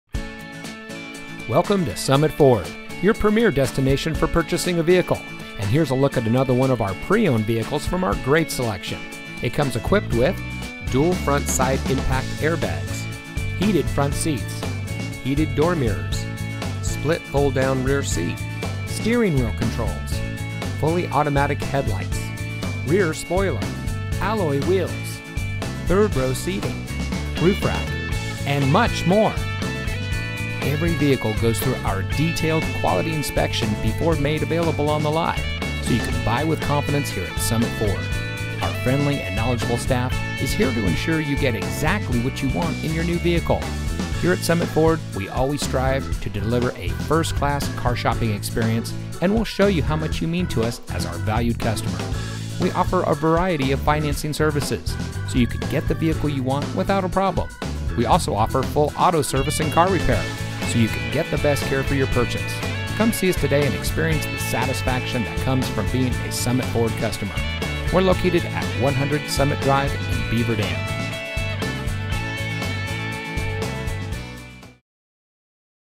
2012 Ford Explorer Limited Sport Utility Used. walk around for sale in Beaver Dam, Wisconsin
2012 Ford Explorer Limited Sport Utility Stock Number: 857XB Vin:1FMHK8F84CGA13429